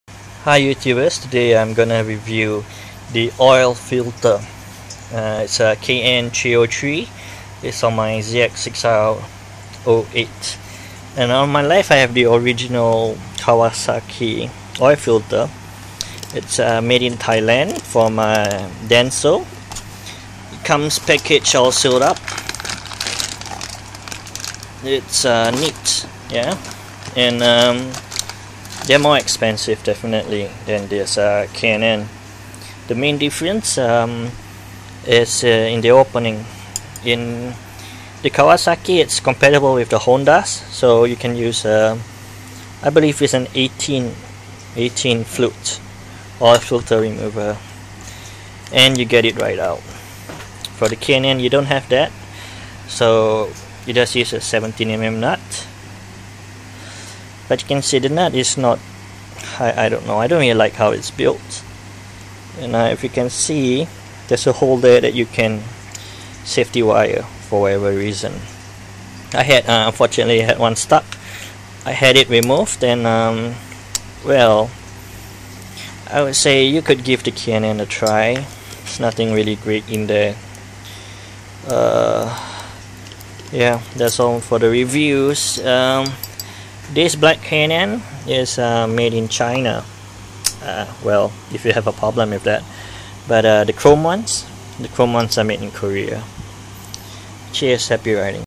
Kn-303 & Kawasaki Oil filter review
A simple comparison between the kawasaki oil filter and the k&n 303 oil filter for the zx6r2008